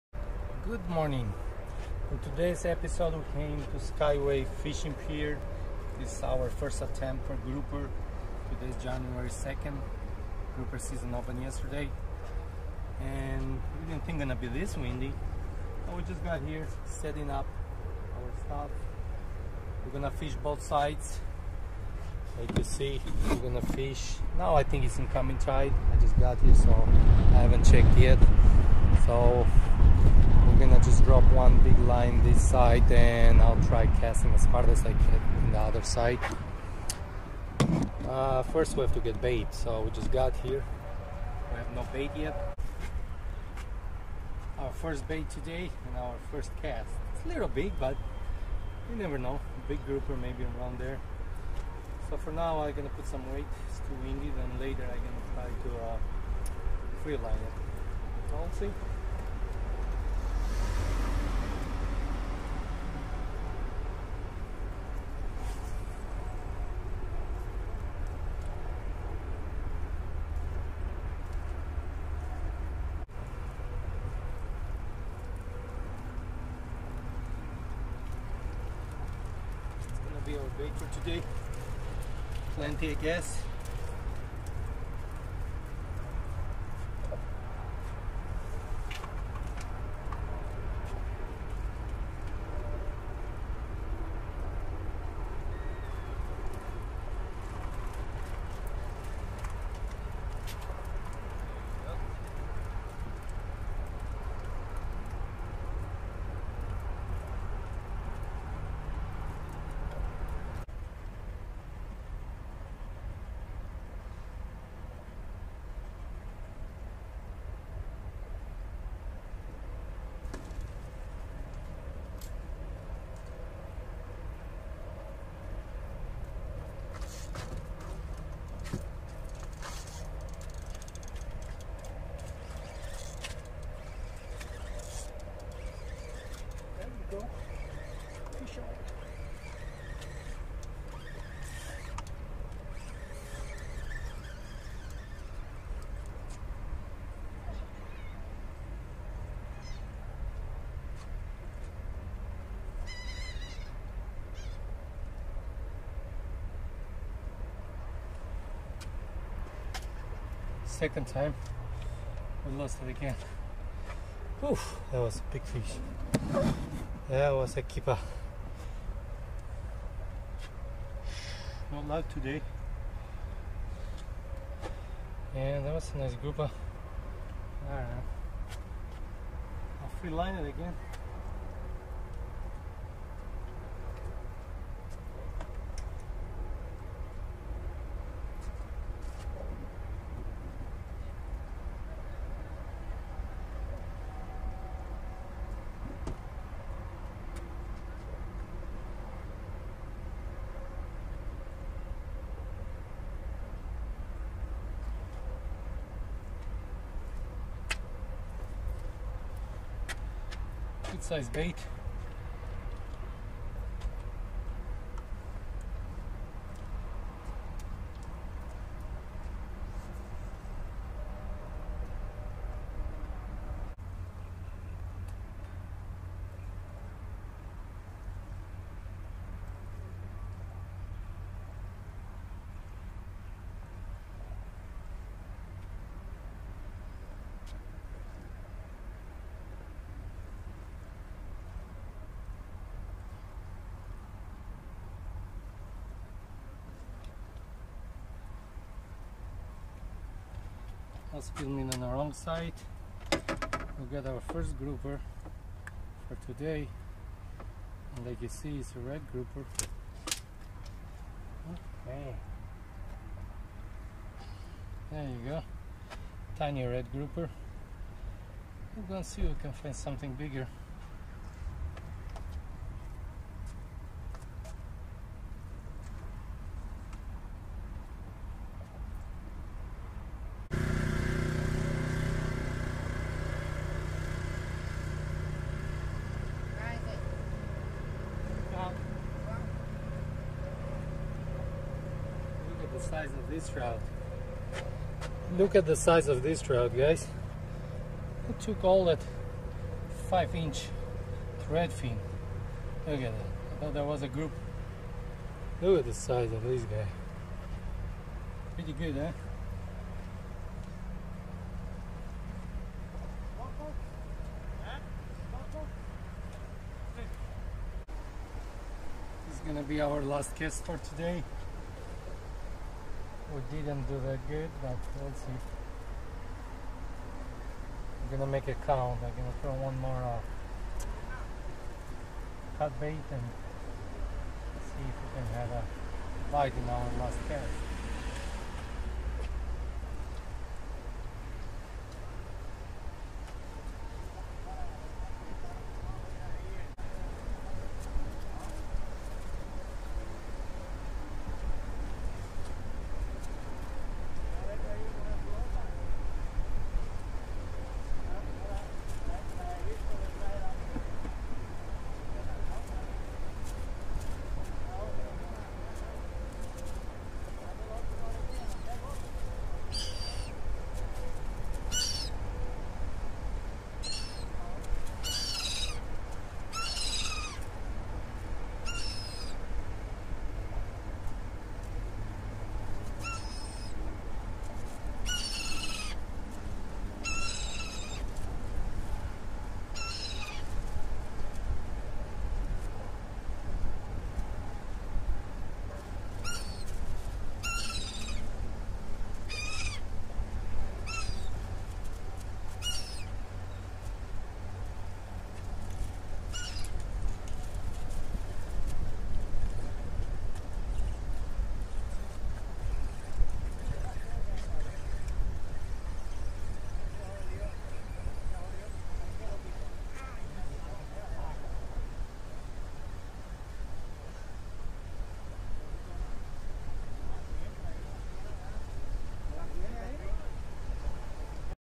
Grouper fishing Skyway south Fishing Pier.
Grouper fishing Skyway south Fishing Pier.  Our first attempt grouper fishing for this season at Sunshine Skyway south Pier. Bite was pretty good i had couple big bites lost few grouper, you can't catch them all. It was a good day to go fishing, we'll go back to try another time. Fish on Channel.